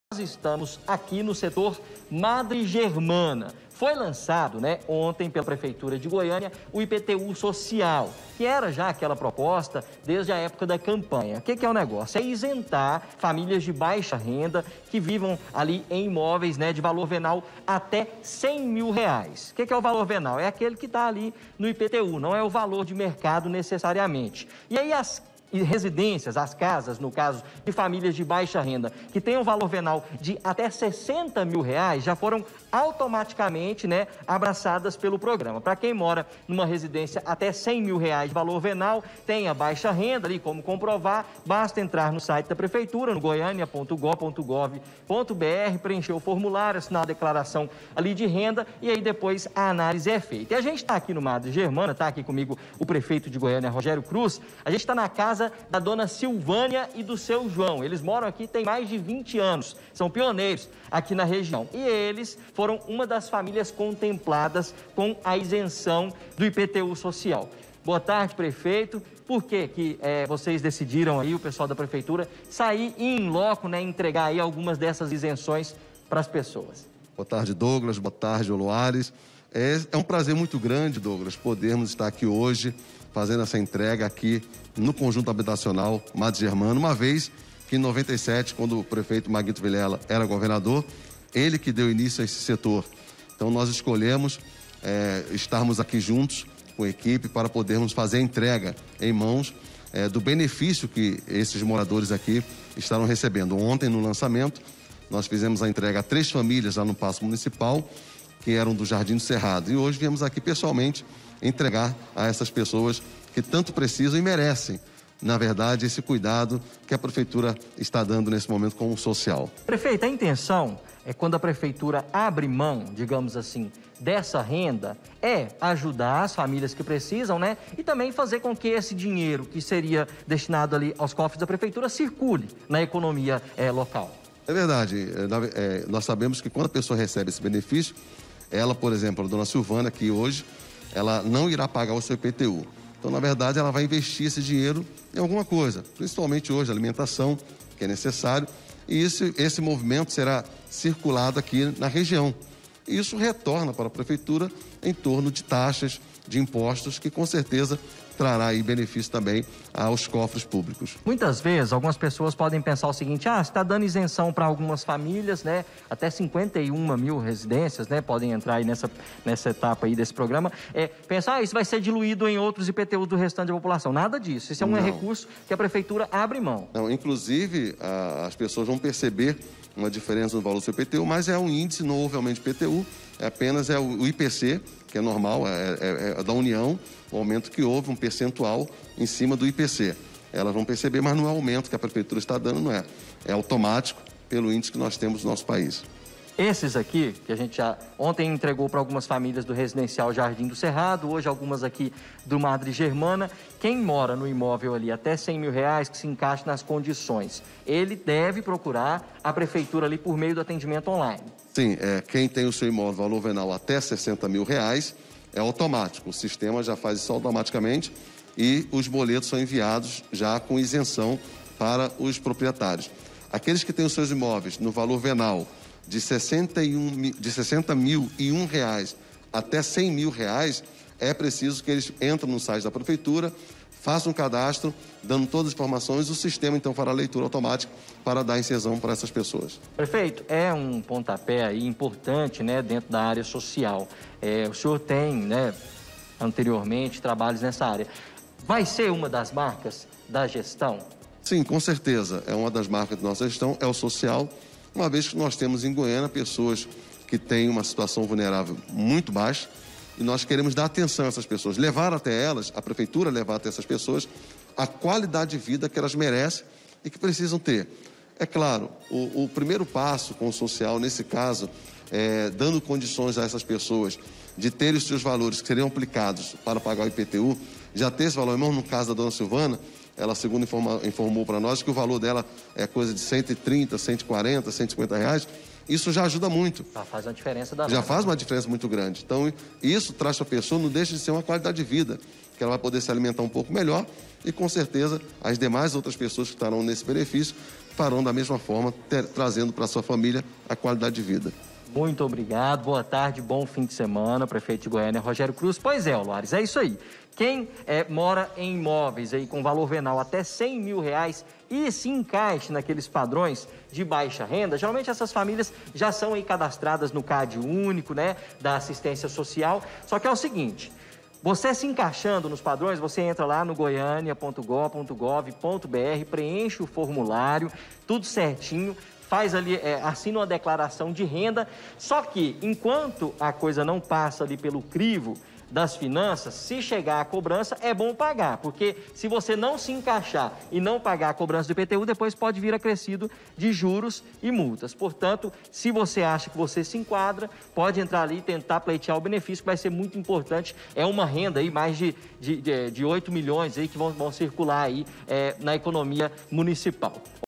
IPTU SOCIAL: BOLETOS QUITADOS SÃO ENTREGUES PARA MORADORES
MOCHILINK DOUGLAS BRANQUINHO. BOLETOS QUITADOS DO IPTU SOCIAL SÃO ENTREGUES PARA MORADORES DO MADRE GERMANA II.